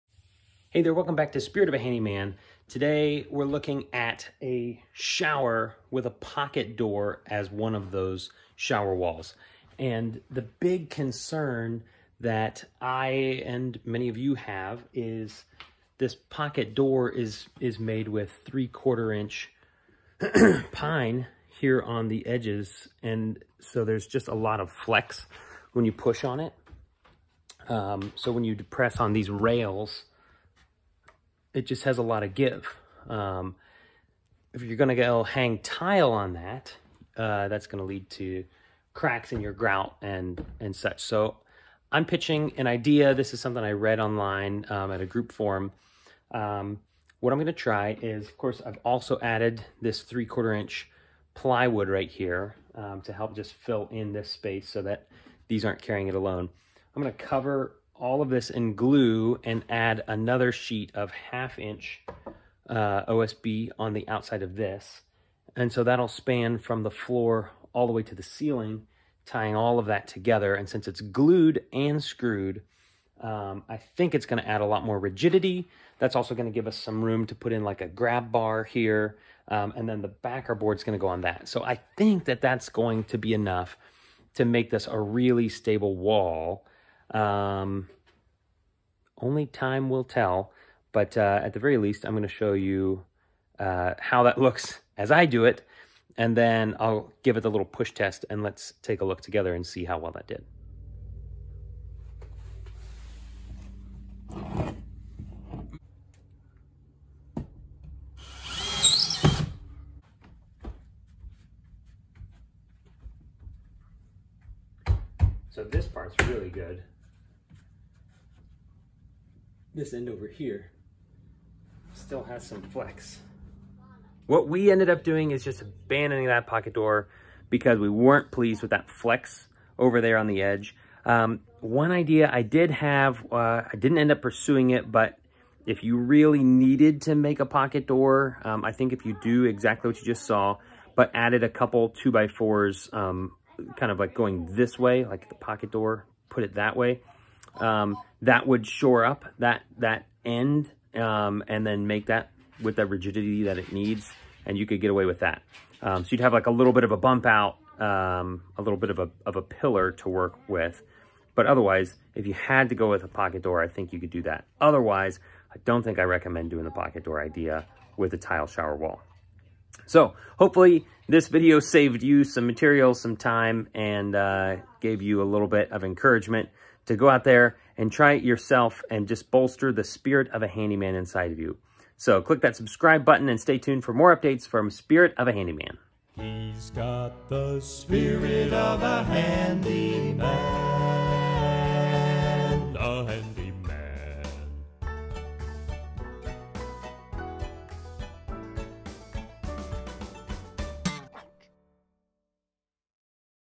Ideas for Framing a Pocket Door Wall for Tile Shower
Ideas for Framing a Pocket Door Wall for Tile Shower. The biggest concern with installing a pocket door in your shower wall is deflection. How do you keep the grout lines from cracking when you lean on the tile wall? In this video, I explore an option for adding 1/2" OSB to the wall with marginal success. Watch this video before trying to tackle your own tile shower on a pocket door.